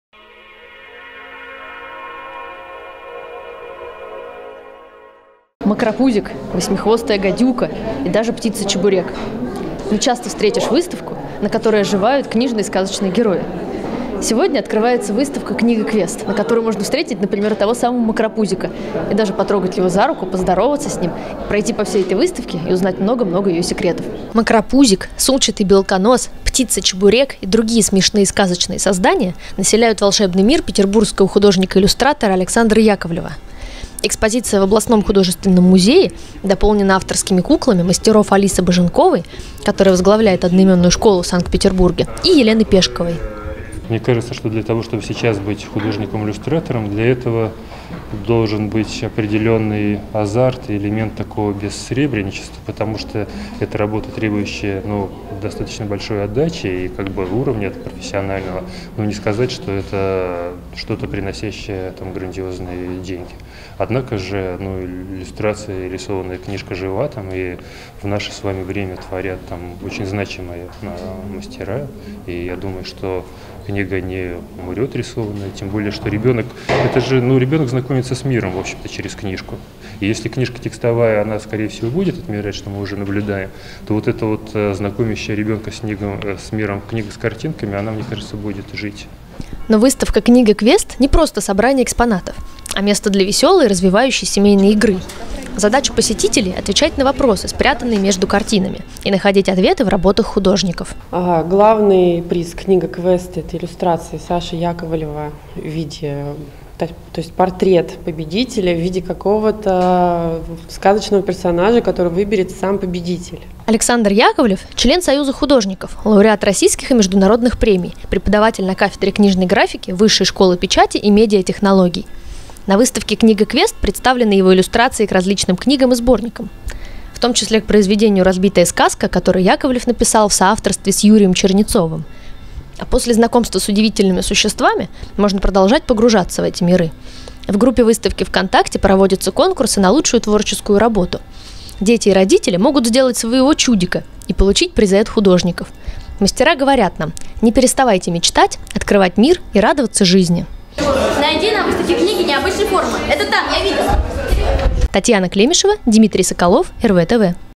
Выставка КнигаКвест в Рязани.РВ ТВ
В Рязанском художественном музее открылся выставочный проект-игра для детей и взрослых современных художников из Санкт-Петербурга, представляющих книги, кукол, иллюстрации и анимацию